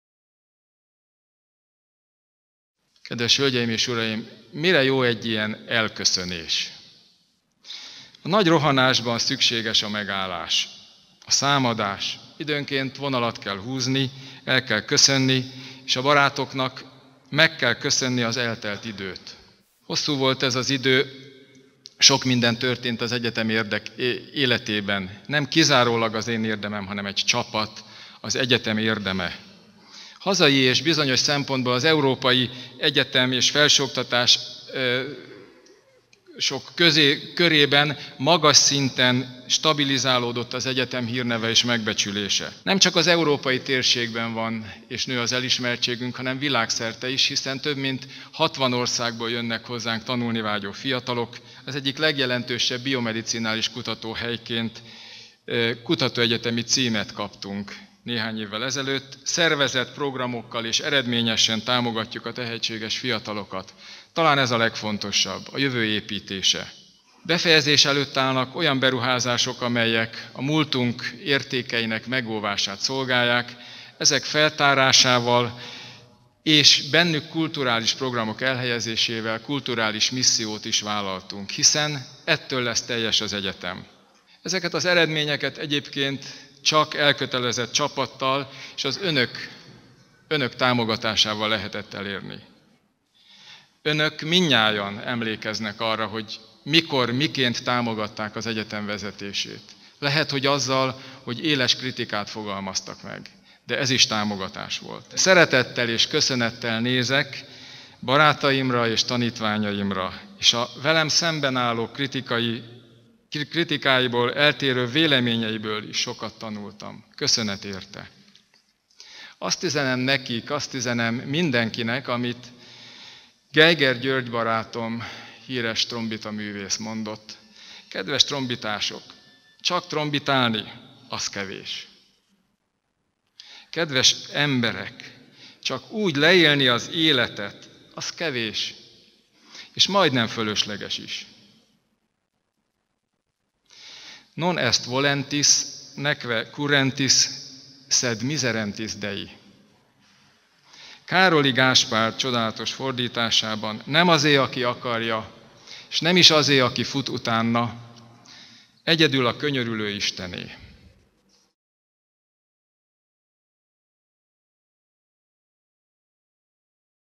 Elbúcsúzott Dr. Tulassay Tivadar, a Semmelweis Egyetem leköszönő rektora
Kilenc év után leköszönt tisztségéről Dr. Tulassay Tivadar. Ahogyan beszéde elején -- zárásként is Pál apostol leveleiből idézett: "Non est volentis, neque currentis, sed miserentis Dei". Ez Károli Gáspár fordításában így hangzik: „Nem azé, aki akarja, s nem is azé, aki fut (utána), egyedül a könyörülő Istené".